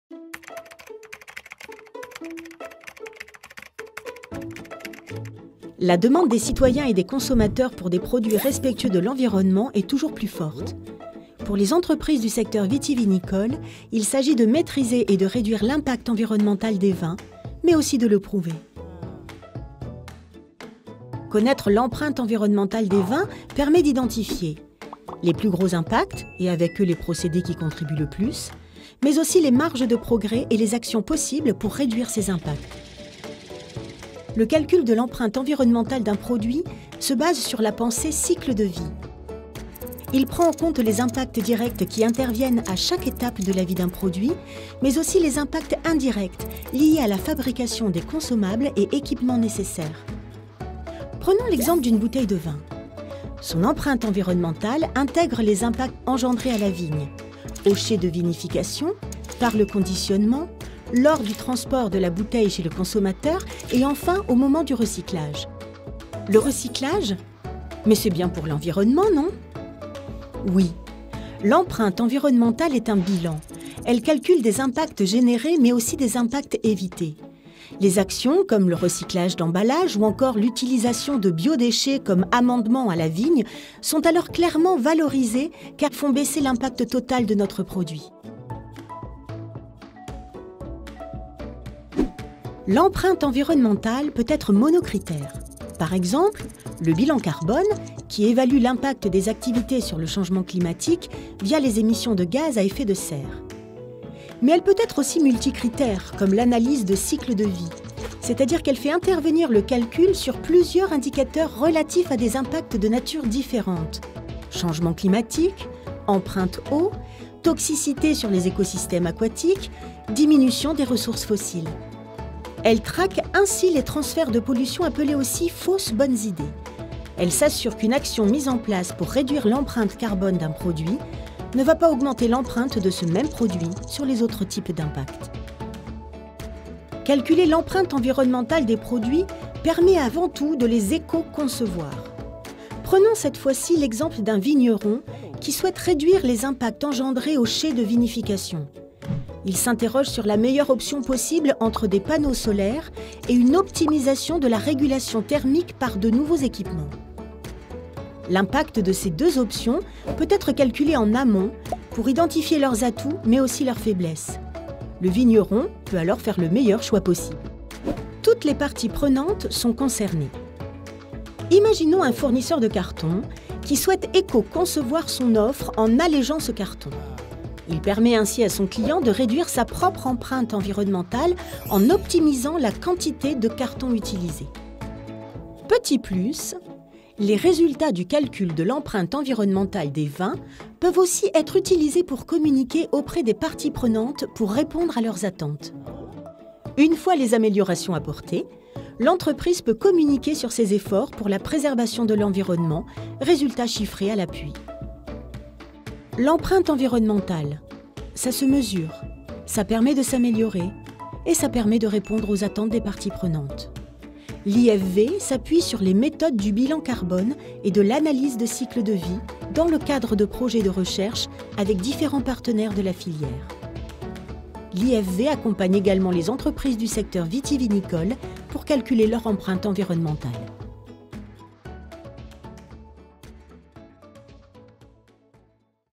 Entreprises du vin : pourquoi et comment connaître son empreinte environnementale ?
Le calcul de l'empreinte environnementale d'un produit se base sur la pensée cycle de vie, mais qu'est ce que c'est ?

La demande des citoyens et des consommateurs pour des produits respectueux de l'environnement est toujours plus forte.
Pour les entreprises du secteur viti-vinicole, il s'agit de maîtriser et de réduire l'impact environnemental des vins. Mais aussi de le prouver.
Connaître l'empreinte environnementale des vins permet d'identifier :
- les plus gros impacts (et avec eux les procédés qui contribuent le plus) mais aussi
- les marges de progrès et les actions possibles pour réduire ces impacts.